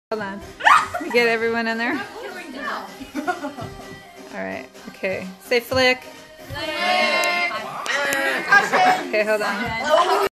Teen Night at Our House ~ Say PHLiC!
Flo hosted a gathering for the teens in our homeschooling group.